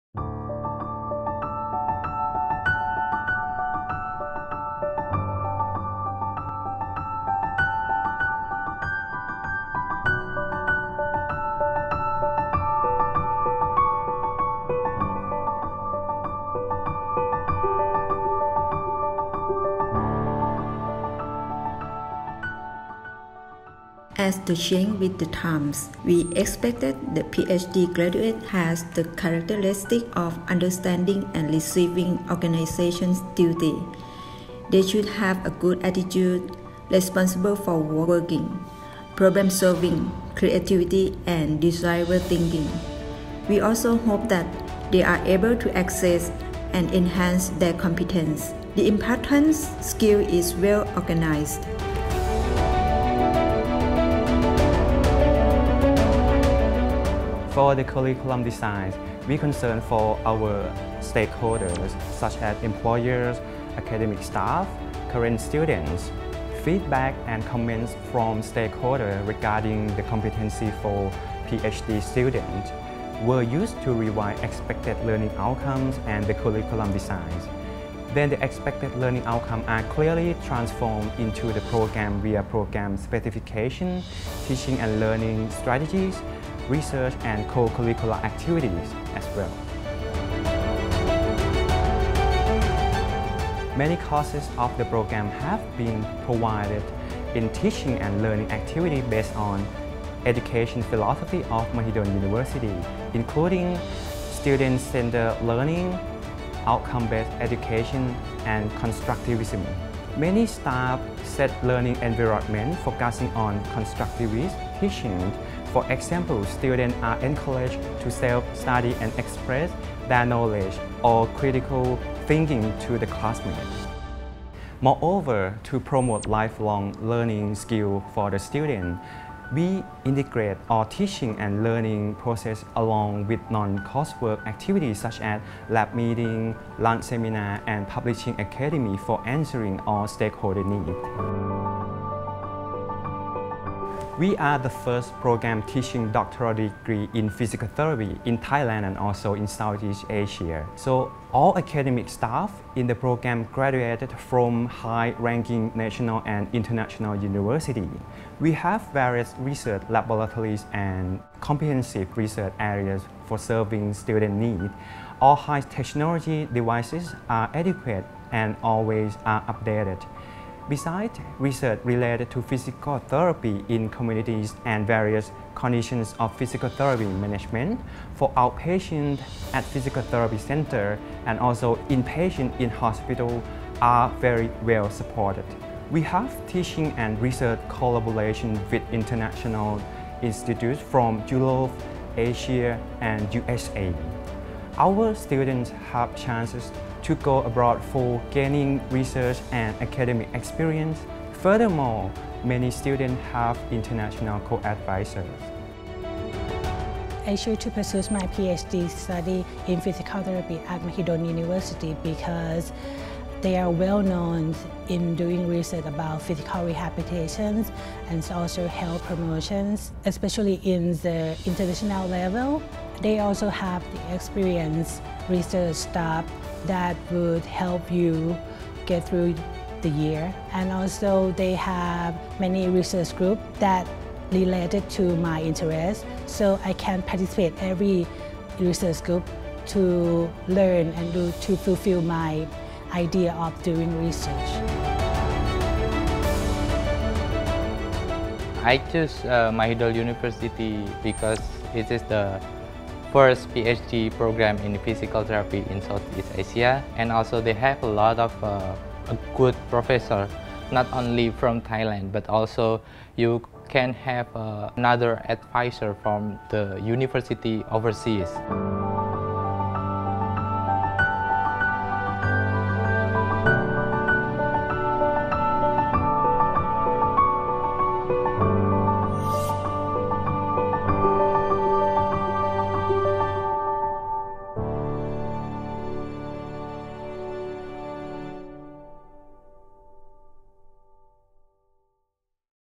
Doctor of Philosophy Program in Physical Therapy (International Program), MU
163rd AUN-QA Assessment at Programme Level
Doctor of Philosophy Program in Physical Therapy (International Program)
Faculty of Physical Therapy